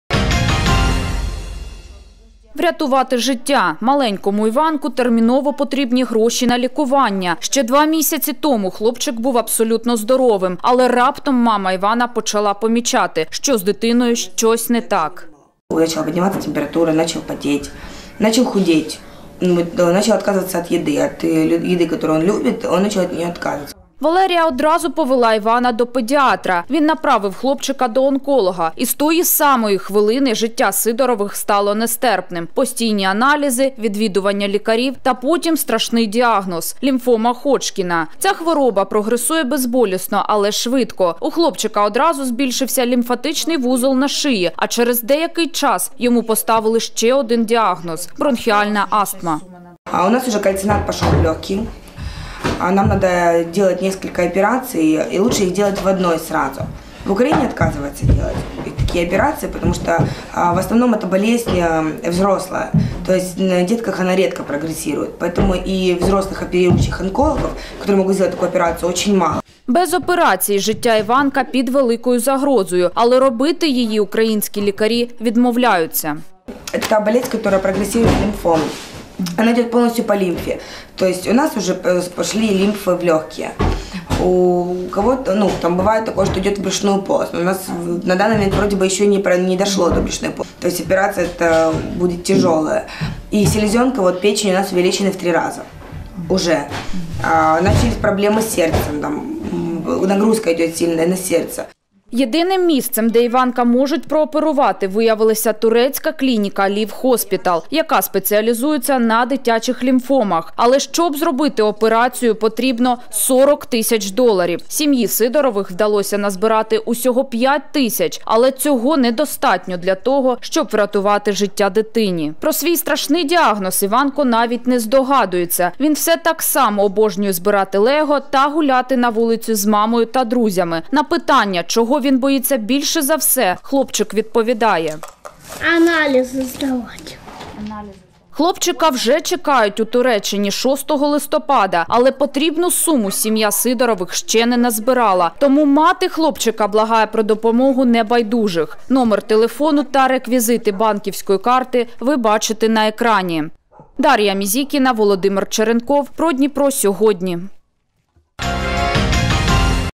Дитині потрібна допомога
Врятувати життя. Маленькому Іванку терміново потрібні гроші на лікування.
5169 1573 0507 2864 Сидорова Валерія Олегівна